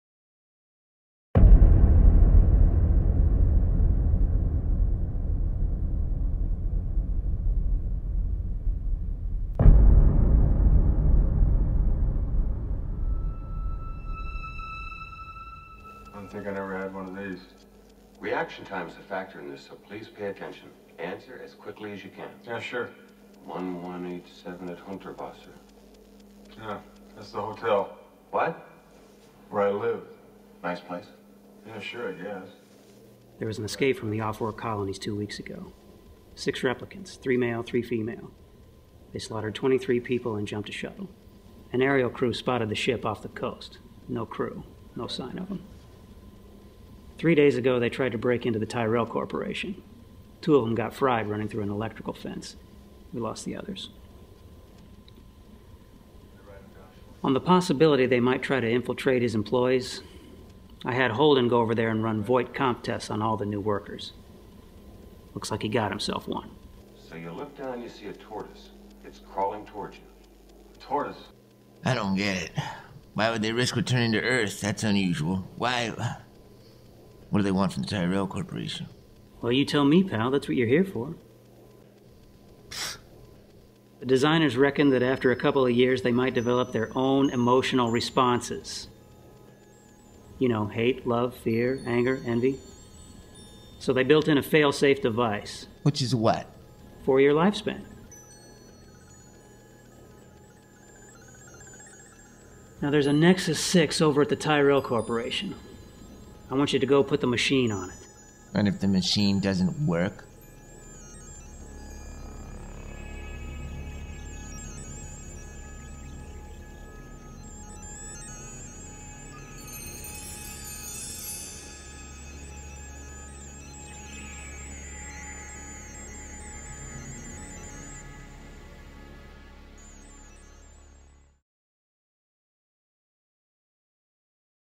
Blade Runner GH1
I recreated a scene from "Blade Runner" which has some of my favorite lighting. I shot it with my Panasonic GH1. And, in a casting coup, Speed Levitch plays Deckard. You may know Speed from films like "The Cruise", "Waking Life", and from the cartoon series "Stroker & Hoop".